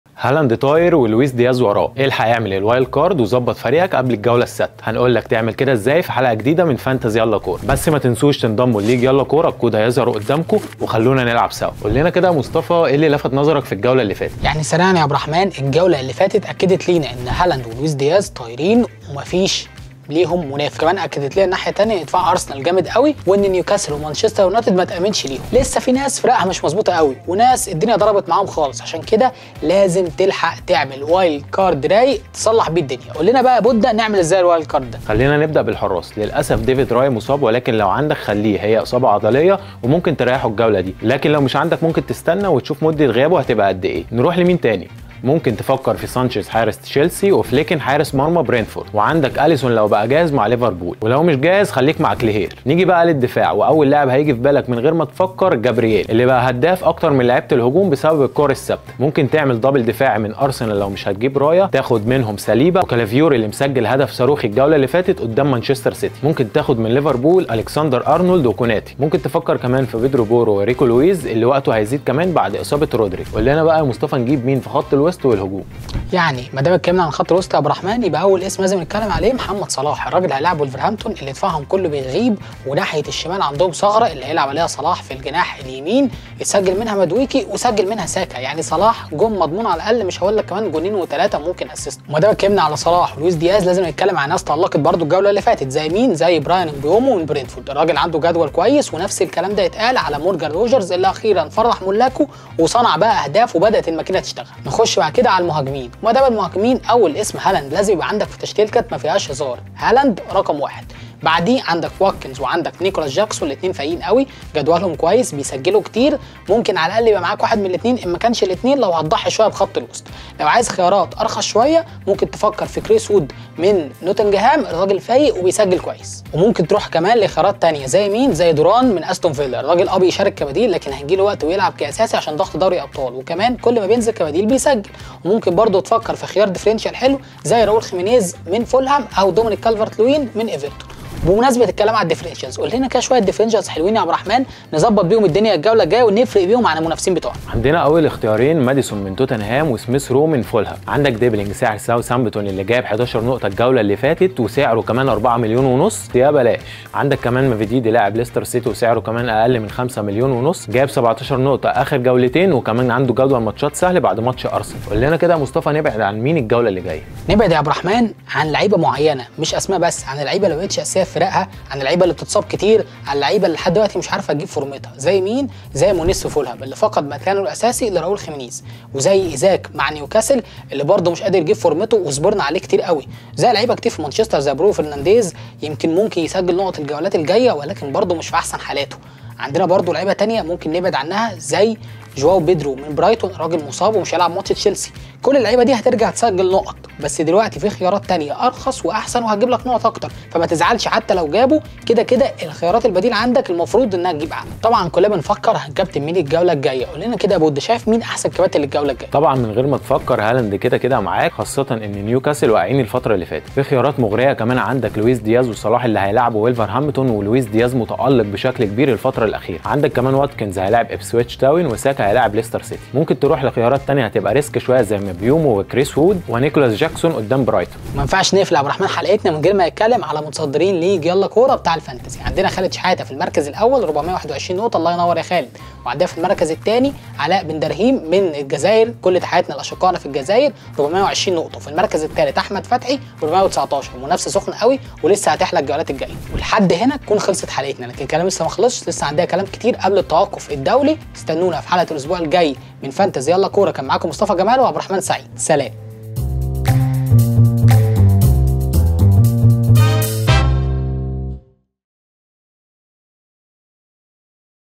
لو بتفكر تفعّل Wild Card تعالى نقولك ممكن تعمل ايه.. ومين اللاعيبة اللي تبعد عنهم خالص
فانتازي_يلاكورة مع مصطفى جمال وعبدالرحمن سعي